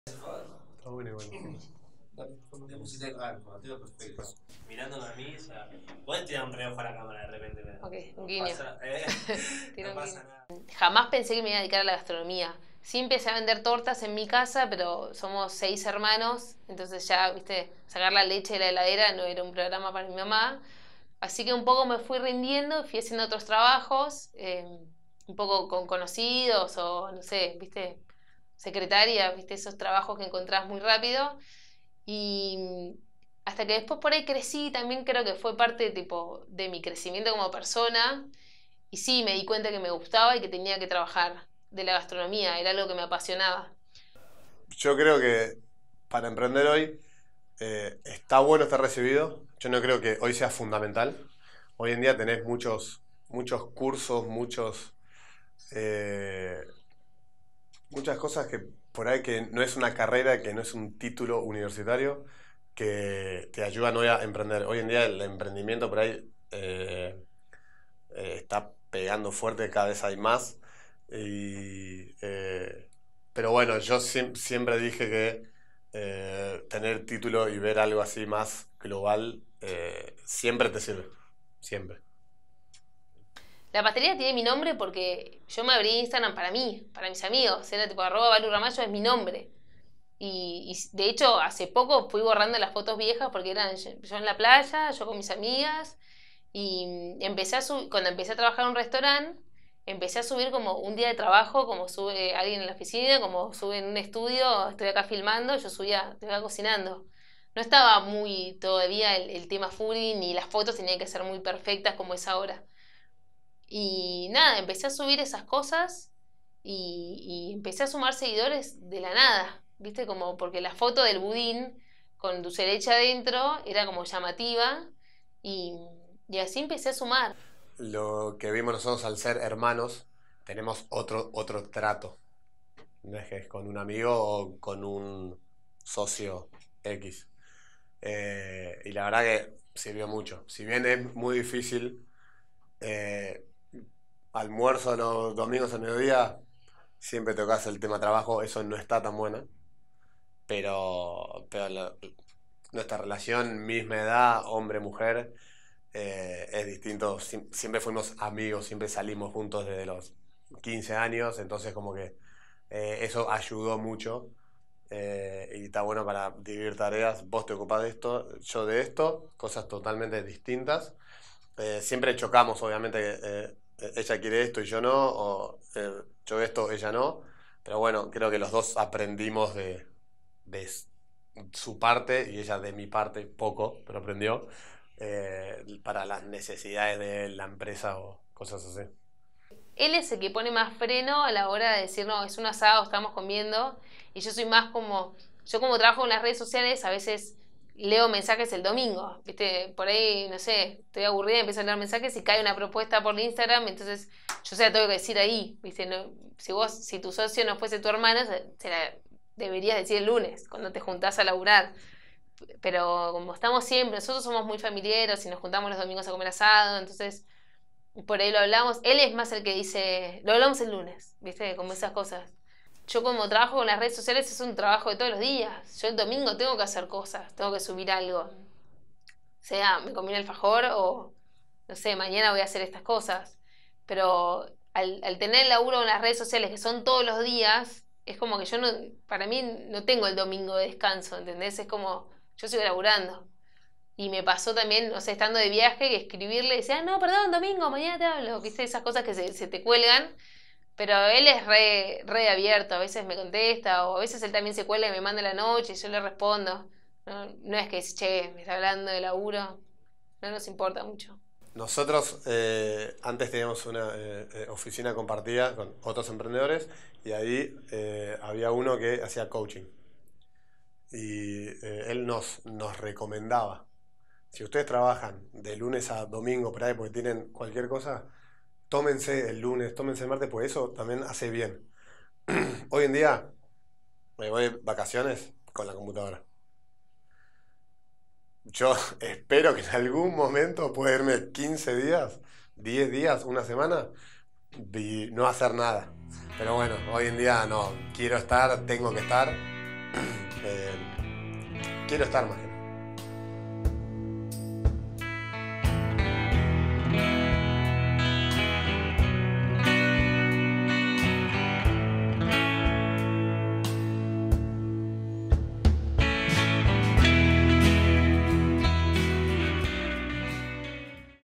Caco y Valu Ramallo: Valu Ramallo
Emprender en familia: la apasionante historia de los hermanos Ramallo

Emprendar, el hub de emprendedores.